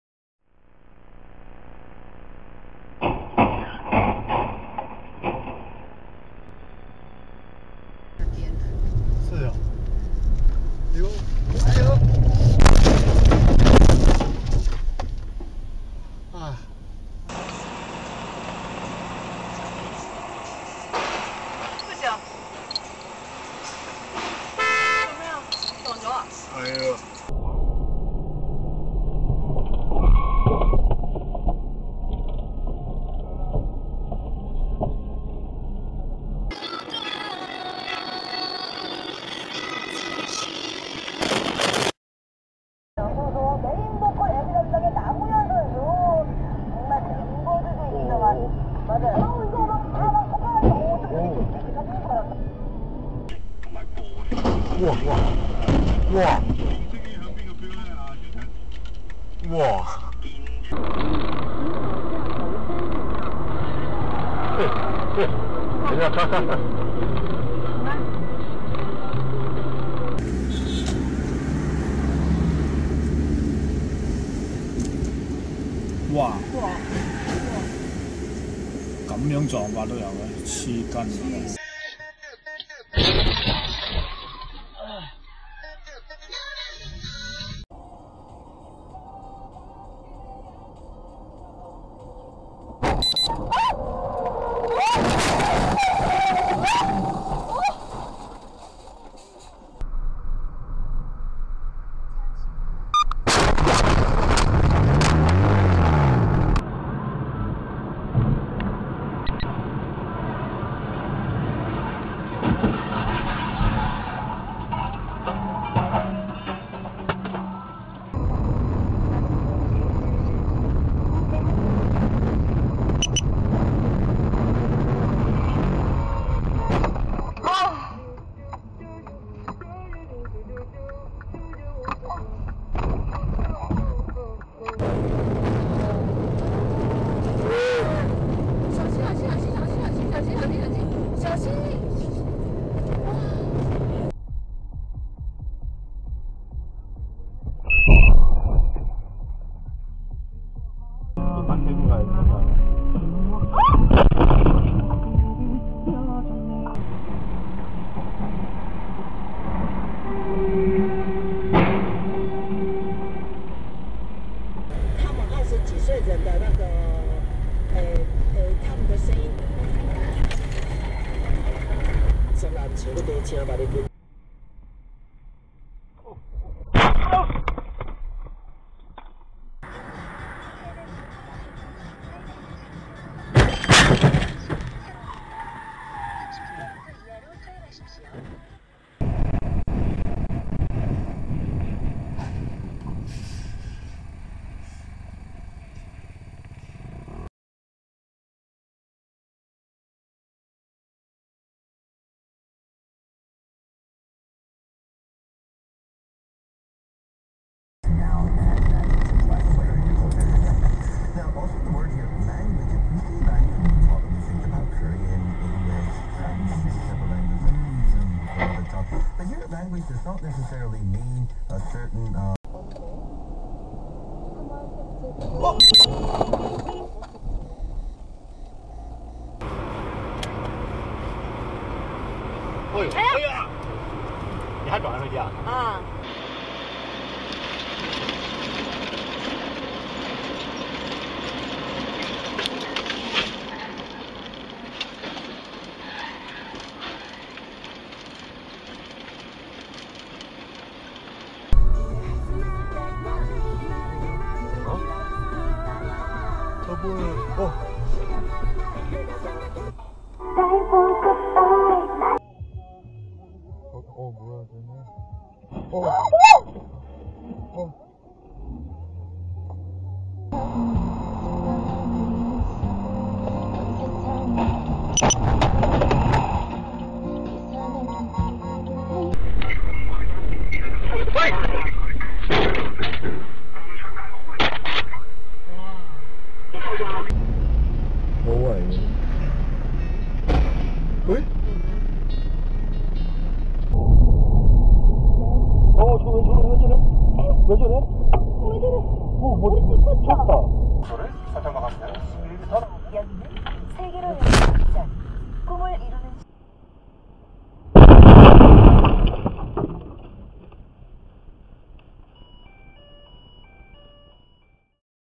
【交通事故映像集2014】ドライブレコーダー アジアでの事故瞬間 (2)
ビデオ2014でキャプチャ自動車事故. 【交通事故映像集2014】ドライブレコーダー　アジアでの事故瞬間.
Asia Car Crash Compilation 2014. Driving in Asia!

Check out the latest dashcam accidents videos and accident compilations from around the web